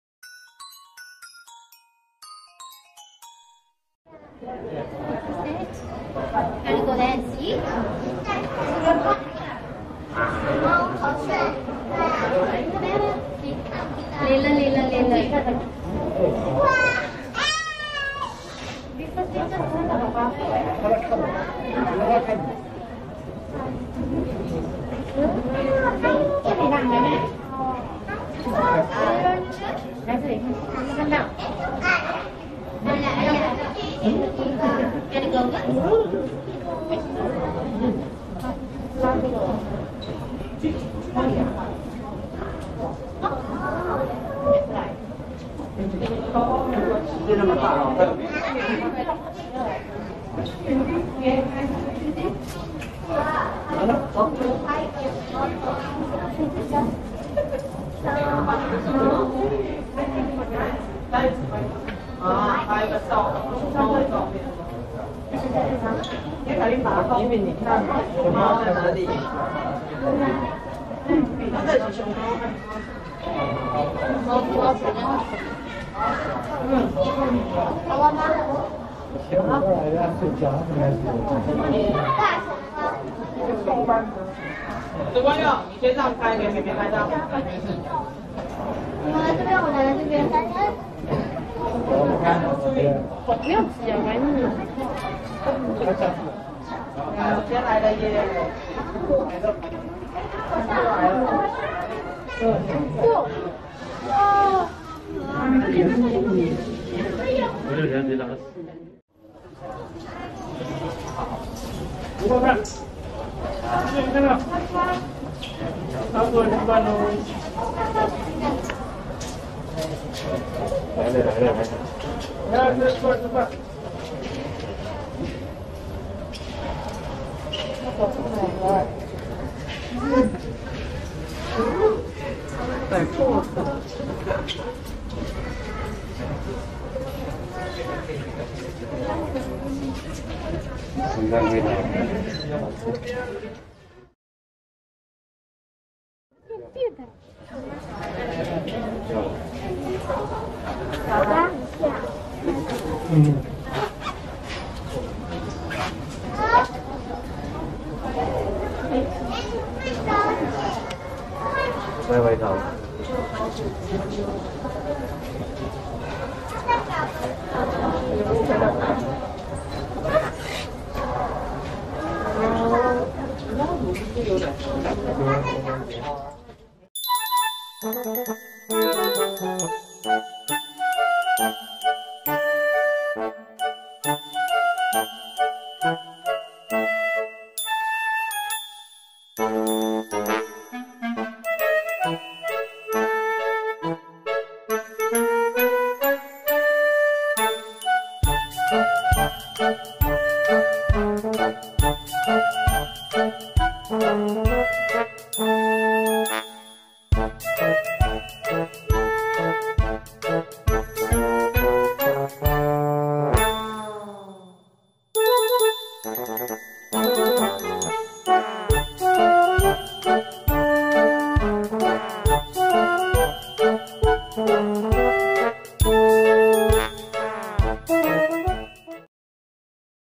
圓圓吃完窩窩頭站木架上許久,站窗前看著遊客嘴微張,張耳聆聽,沒吃筍上棲架休息|Giant Panda Yuan Yuan ,圆圆,貓熊,大貓熊,大熊貓|p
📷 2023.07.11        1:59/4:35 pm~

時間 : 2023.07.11
地點 : 台北市立動物園
天氣 : 32°C 陰天
圓圓出生 :  2004年08月30日
性別 : ♀

Filmed on Jul.11,2023
Location : Taipei Zoo, Taipei, Taiwan
Yuan Yuan Birth Date : Aug. 30th, 2004
Gender : ♀